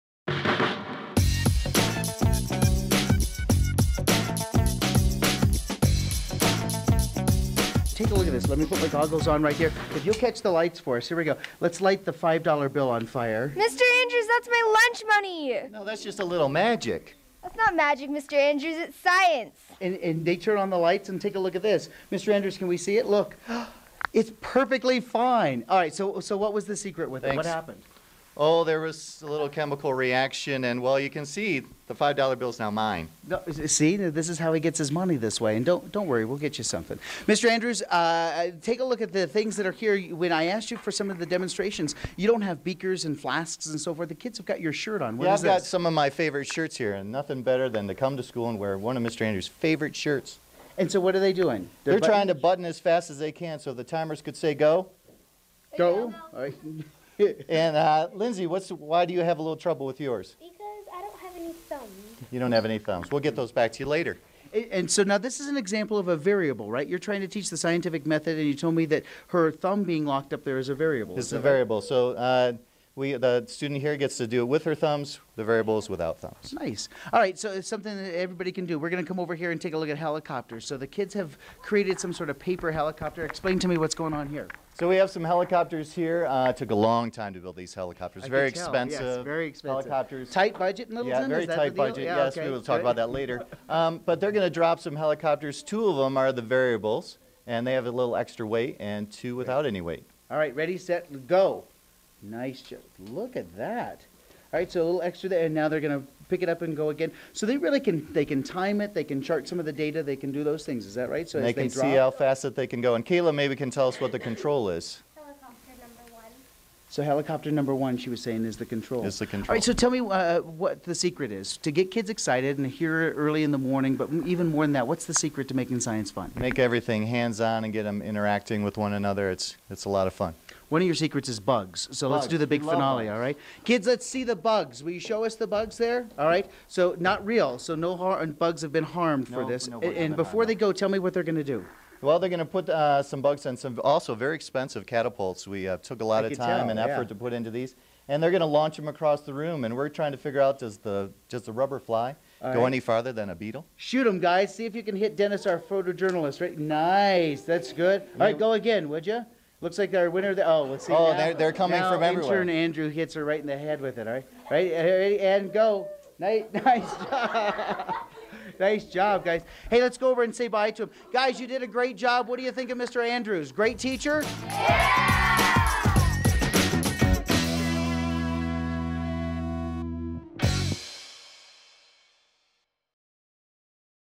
Teacher Appreciation - Tom Andrews
Steve heads to Goddard Middle school in Littleton, CO to meet with Tom Andrews and his students as they share some science secrets.